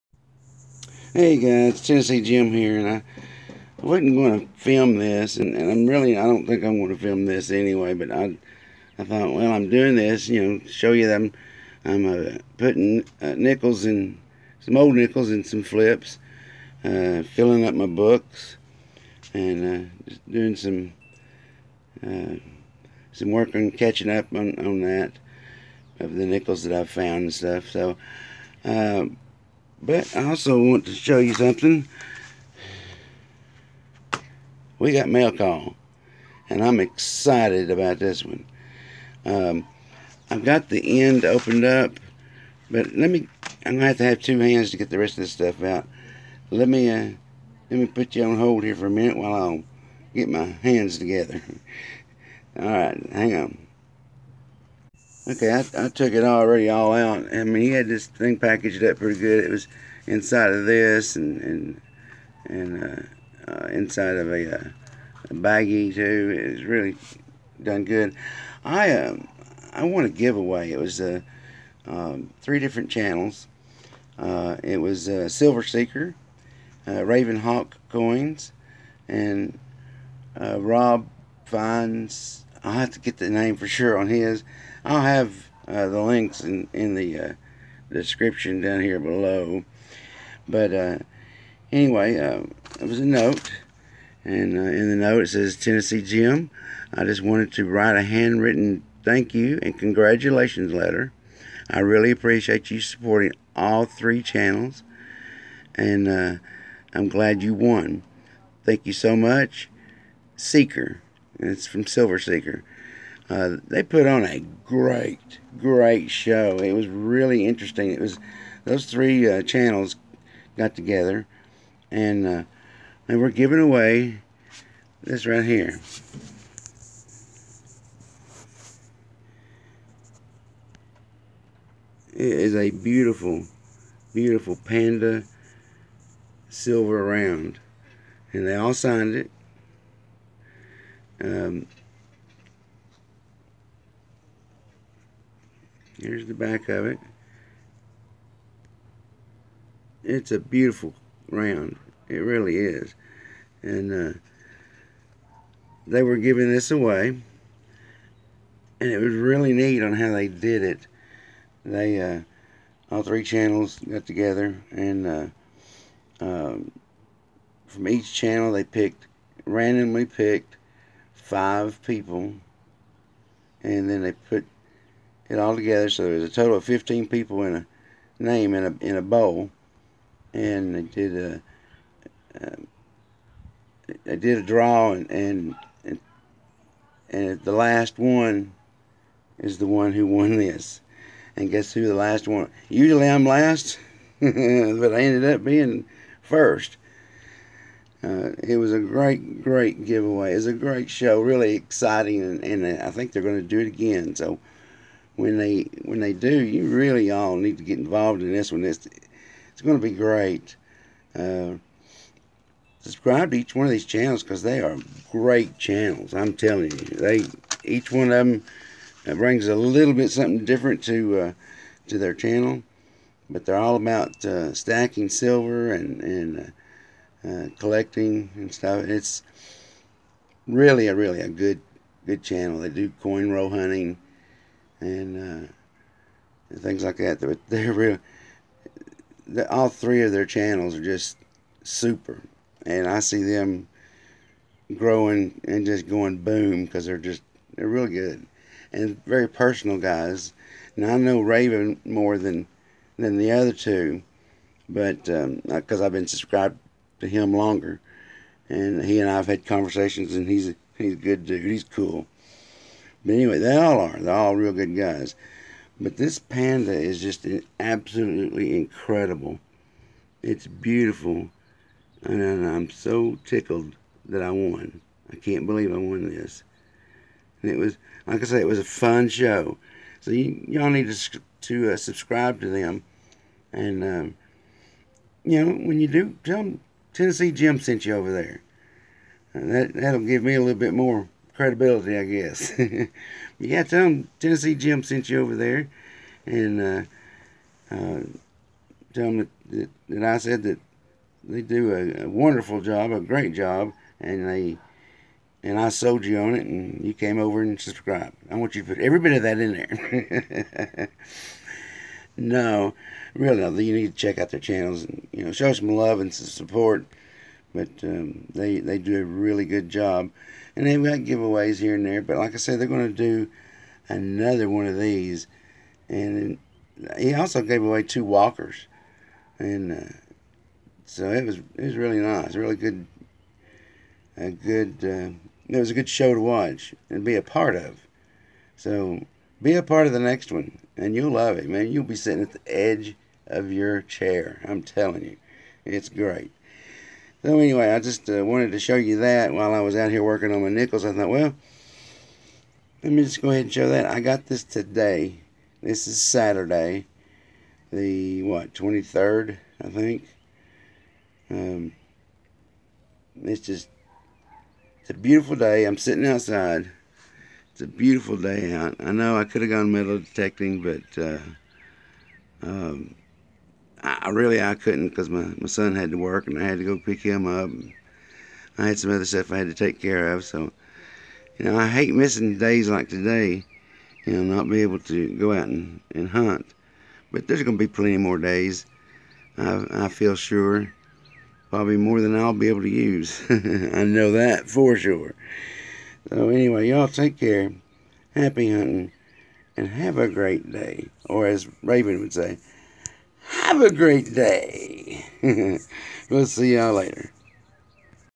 Doing nickels, mail came by with my GAW win from Silver seeker, Raven Hawk coins, RobFinds Treasure
I won a Great GAW from Silver Seeker, Raven Hawk coins and RobFinds Treasure. I won a 2019 Panda silver round.